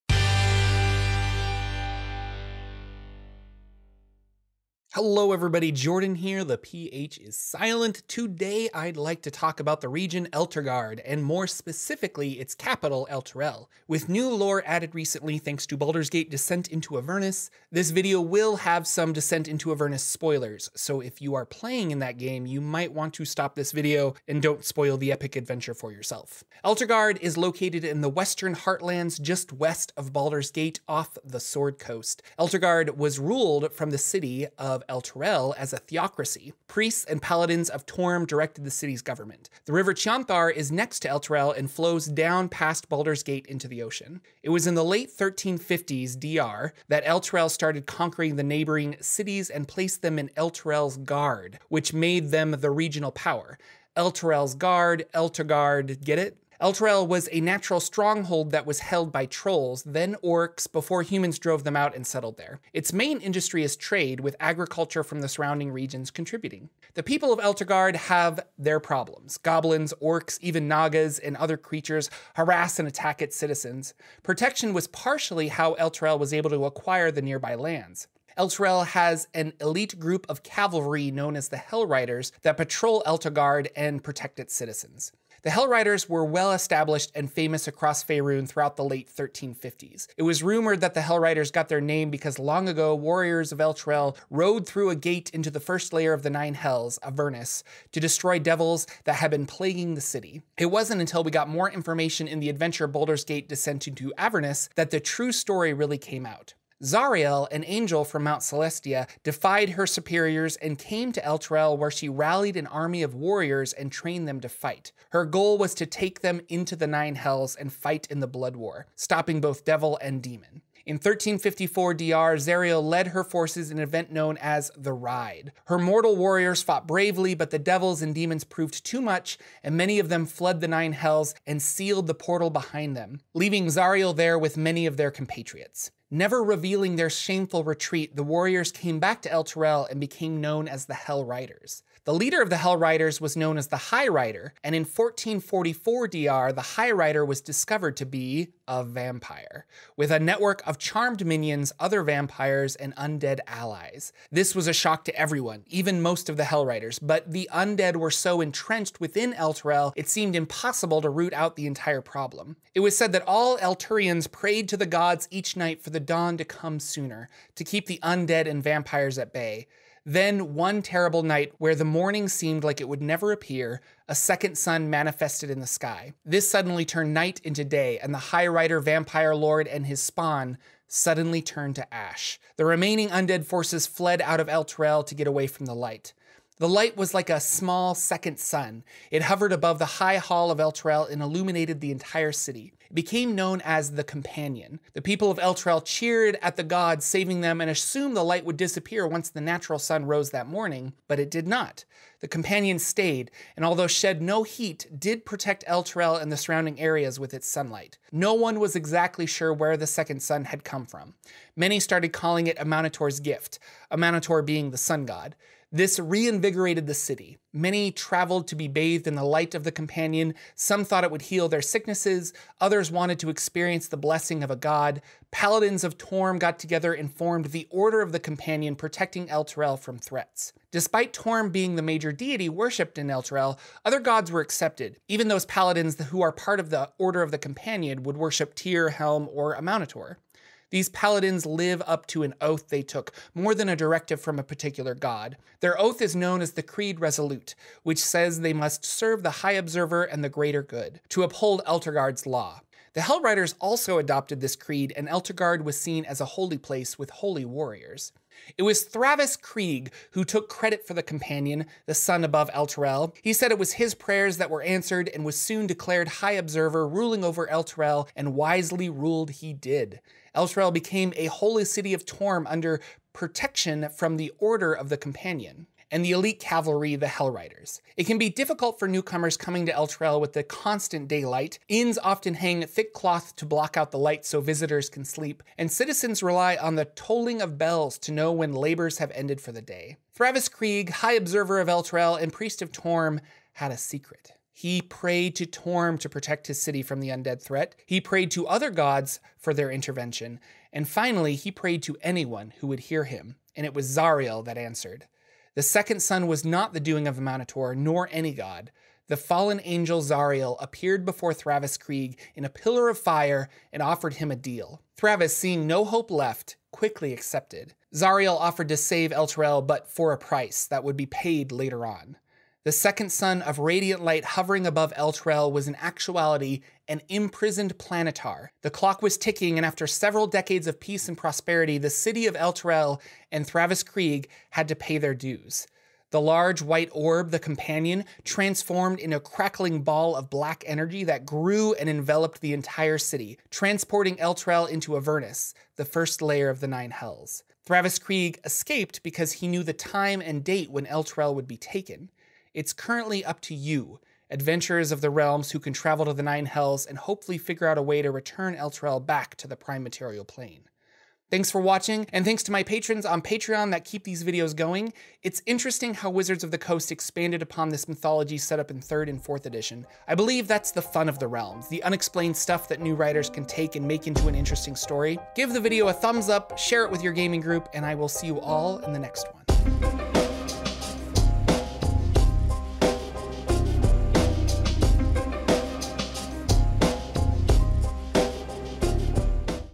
Elturel, the Holy City | D&D Lore
Episode 133! In this video I talk about Elturel and Elturgard, which met a tragic end during the events of Baldur's Gate: Descent into Avernus.

Saturday Morning D&D Show!
Or check it out in Podcast Format! 🎙️

References:
Baldur’s Gate Descent into Avernus
Sword Coast Adventurer’s Guide